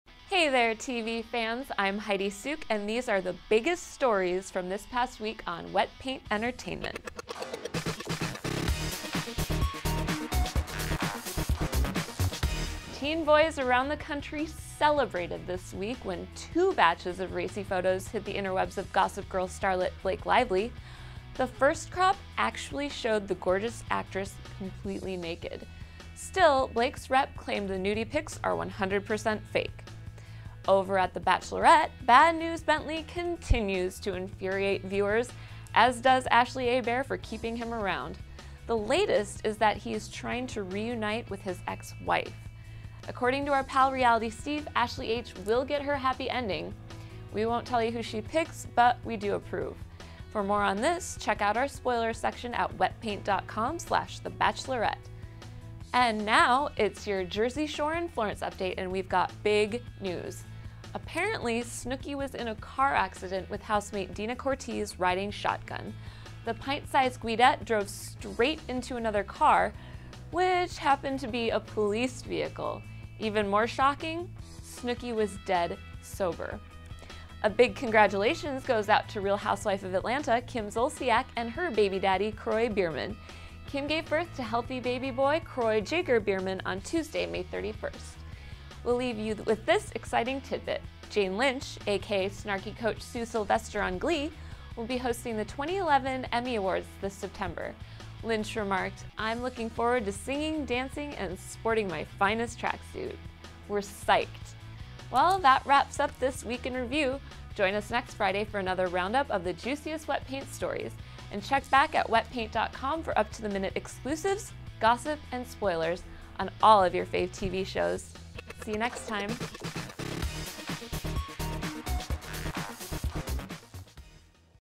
Blake's Nude Pics, Bachelorette Gossip, and Snooki Crashes: Wetpaint's Week in Review, 6/3!
- Visit us for more exclusives, gossip, and spoilers!

Hey there, TV fans! I'm Heidi Suke and these are the biggest news stories from this past week on Wetpaint Entertainment.

Teen boys around the country celebrated this week when two batches of racy photos hit the Interwebs of Gossip Girl starlet, Blake Lively. The first crop actually showed the gorgeous actress completely naked. Still, Blake's rep claimed the nudie pics are 100% fake.

Over at The Bachelorette, Bad News Bentley continues to infuriate viewers, as does Ashley Hebert for keeping him around. The latest is that he's trying to reunite with his ex-wife. According to our pal Reality Steve, Ashley H. will get her happy ending — we won't tell you who she picks, but we do approve! For more on this, check out our spoilers section on Wetpaint.com/The-Bachelorette.

And now, it's your Jersey Shore in Florence update and we've got big news: Apparently, Snooki was in a car accident with housemate Deena Cortese riding shotgun. The pint-sized guidette drove straight into another car... which happened to be a police vehicle! Even more shocking, Snooki was dead sober.

A big congratulations goes out to Real Housewife of Atlanta, Kim Zolciak and her baby daddy Kroy Biermann! Kim gave birth to healthy baby boy Kroy Jagger Biermann on Tuesday, May 31.

We'll leave you with this exciting tidbit: Jane Lynch, aka snarky coach Sue Sylvester on Glee, will be hosting the 2011 Emmy Awards this September. Lynch remarked, "I'm looking forward to singing, dancing, and sporting my finest tracksuit." We're psyched!

That wraps up this Week in Review. Join us next Friday for another roundup of the juiciest Wetpaint stories. for up-to-the-minute exclusives, gossip, and spoilers on all of your fave TV shows.